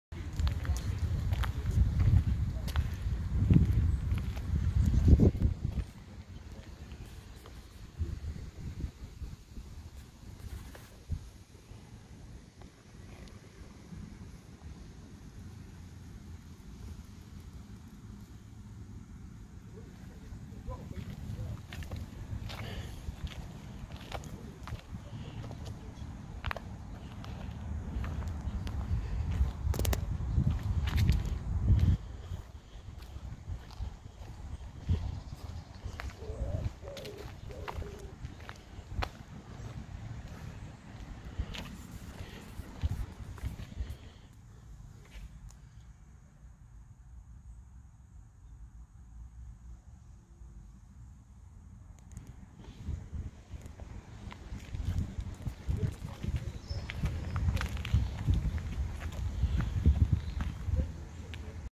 2015 VW caddy van 1.6 tdi in bamboo garden green metallic
Only 20,000 miles . 1 owner in immaculate condition